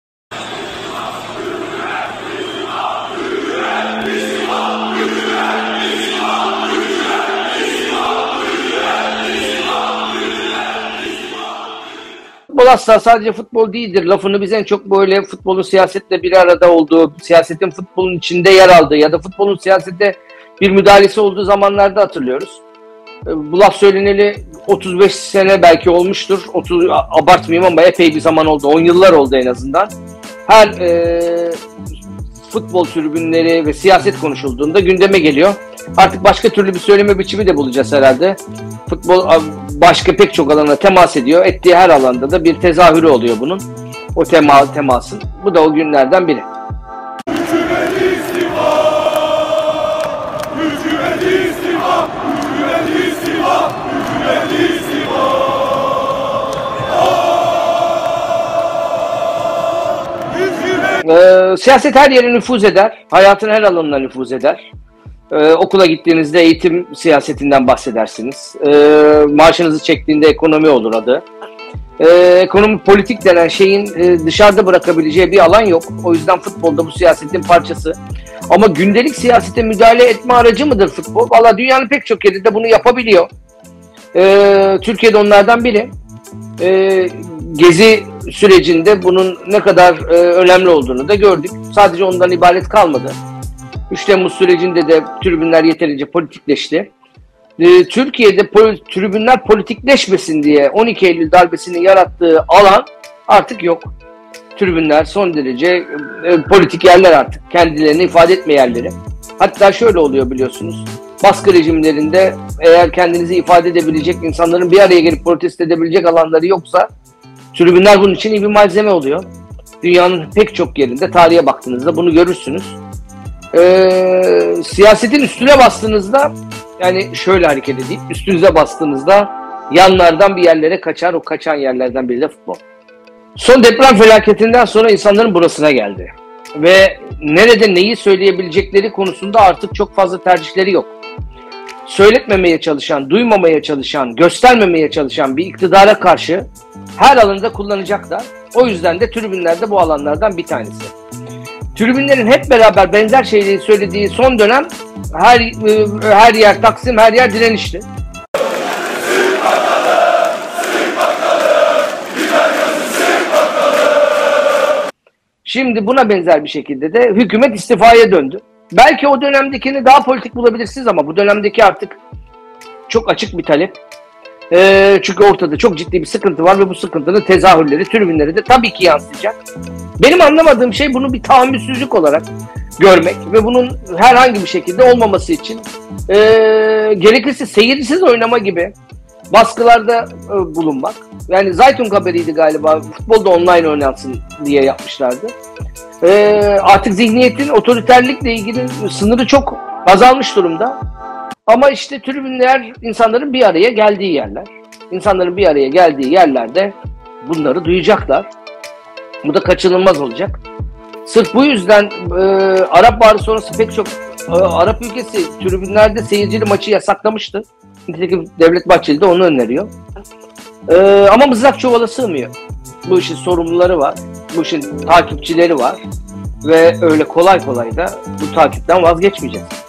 Stadyumlar siyaset yeri midir? | "Hükümet istifa" tezahüratlarını Bağış Erten yorumladı
Kahramanmaraş’ta meydana gelen ve 11 ili etkileyen depremlerin ardından ara verilen Spor Toto Süper Lig, cumartesi günü oynanan erteleme karşılaşmaları ile başladı. Fenerbahçe’nin Konyaspor ile Beşiktaş'ın ise Antalyaspor ile oynadığı karşılaşmalarda, tribünlerden hükümete tepki vardı. Taraftarlar, "Hükümet istifa" sloganları attı.

Spor yorumcusu Bağış Erten, "Hükümet istifa" tezahüratının ifade ettiklerini yorumladı.